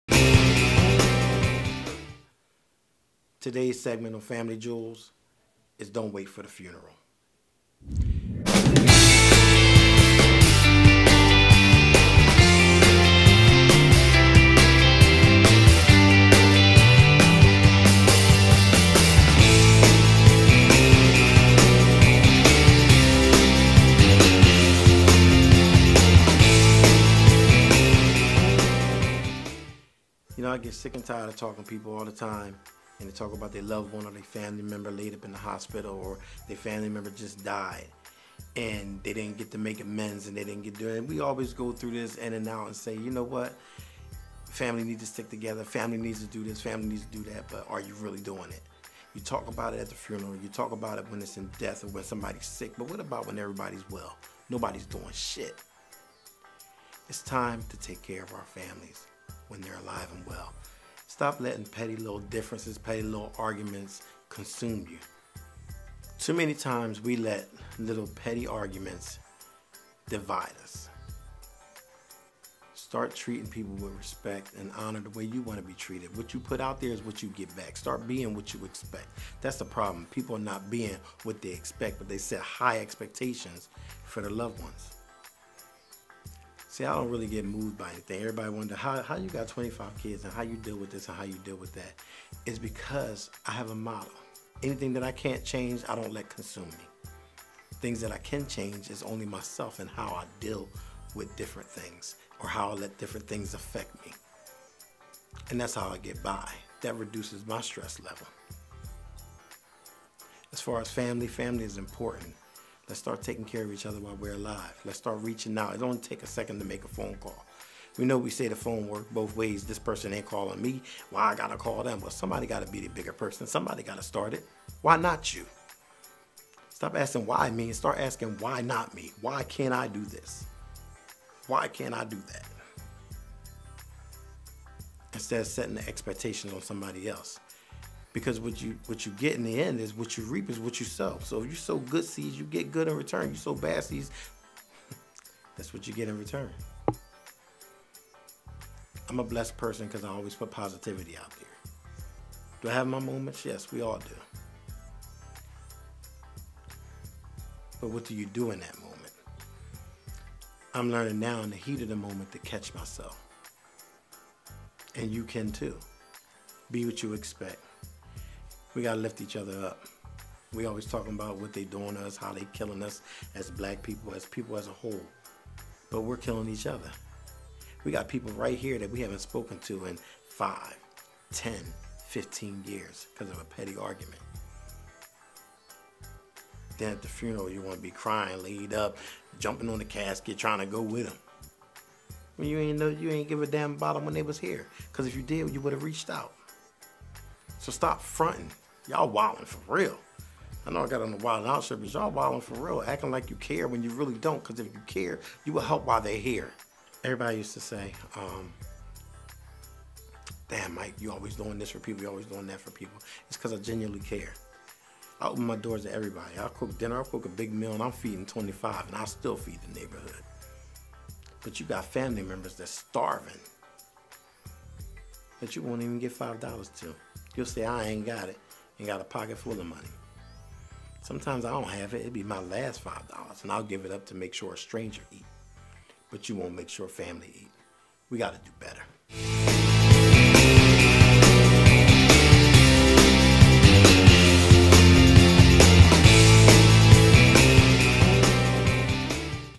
Family Jewels w/ Mykal Coles - Don’t Wait for the Funeral
Mykal Coles speaks on family values.

Father of 25|Husband|Actor|Producer|Singer|
Reality TV|Speaker|Family Life Coach|